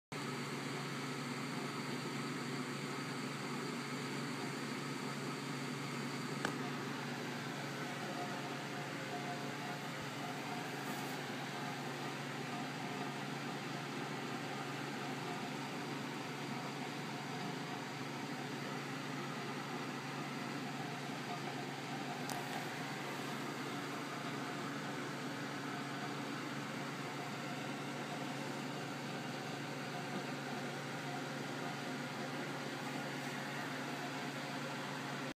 Sensual Siamese Massage?
Siamese cat offers his sibling a  relaxing massage...but there's a payoff...